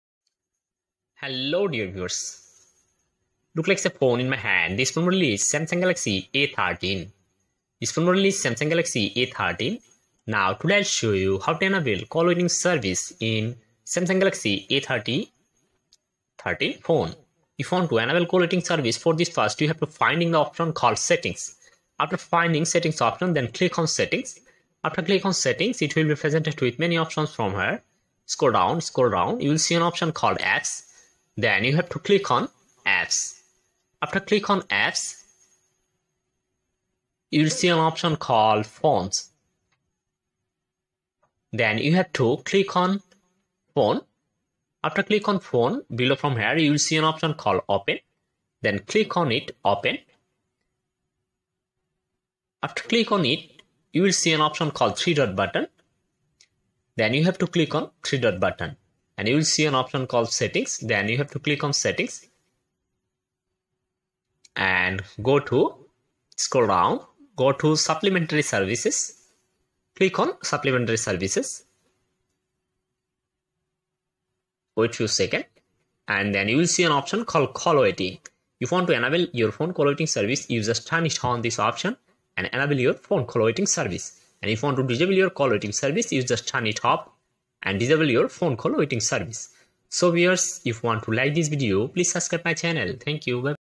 Samsung galaxy A13 call waiting Service Enable // How To Enable Call Waiting in Samsung a13
now today i will show you how to enable call waiting service in Samsung galaxy A13 phone.if you want to enable call waiting service in Samsung galaxy a13 phone for this first you have go to settings and find phone dial pad option go to phone dial pad option and enable your phone call waiting service.